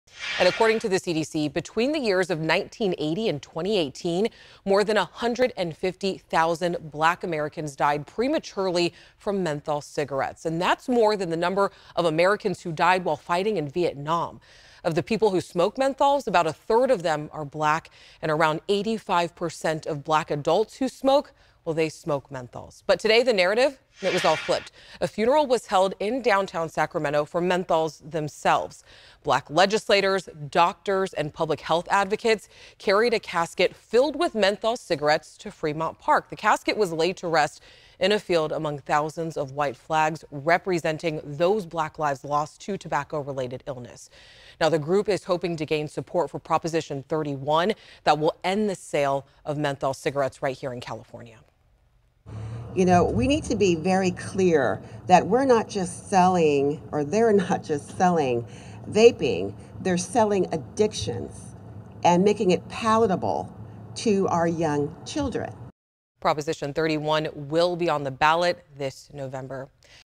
Black lawmakers carry caskets with menthol cigarettes in Sacramento
The casket is meant to represent the Black lives lost to menthol cigarettes and tobacco related illnesses.